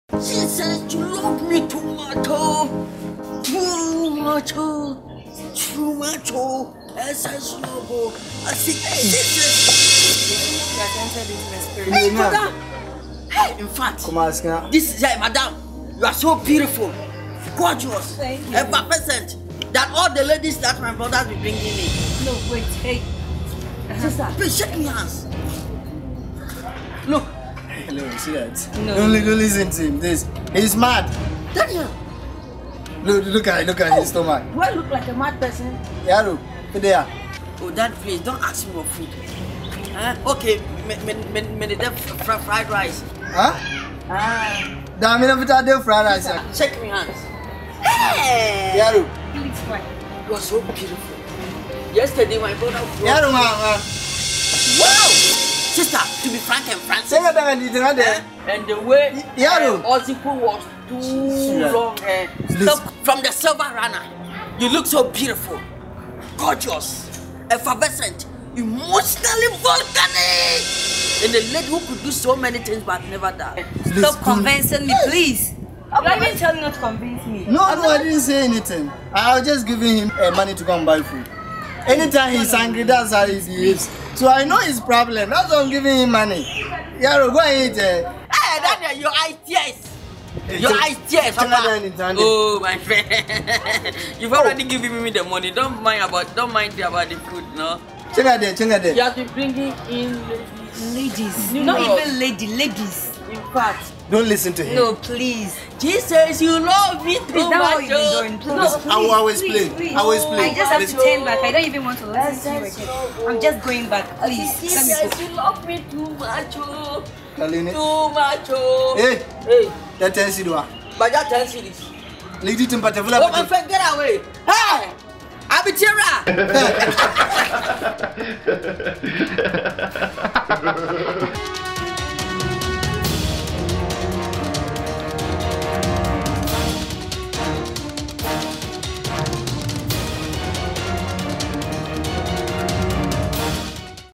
THE CHEATER EXPOSED
DANIEL FAVOR'S new catch left after his brother exposed him of bringing ladies to the house everyday.